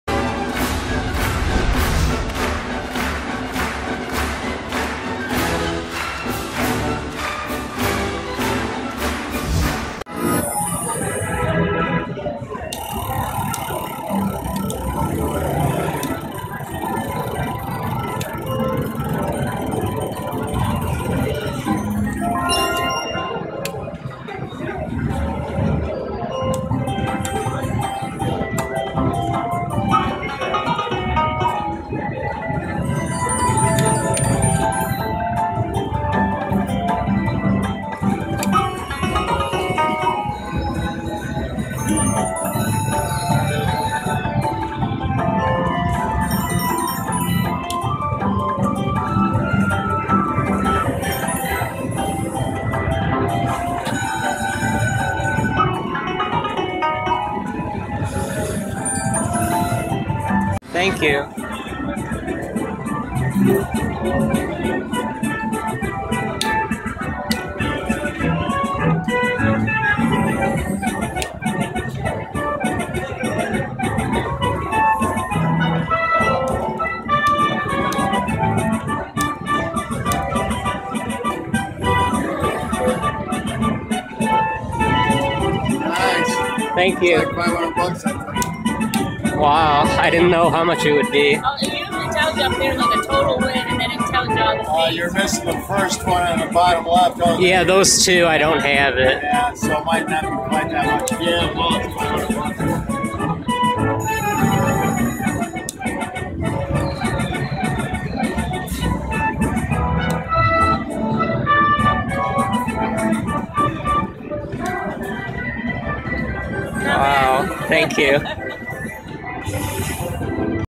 BIG WIN - Wonder 4 Wonder Wheel Wild Panda Bonus & Line Hit Slot Machine
Location: JACK Thistledown Racino, Bet: $2.00
This is the first time I've really played Wild Panda! I definitely will again!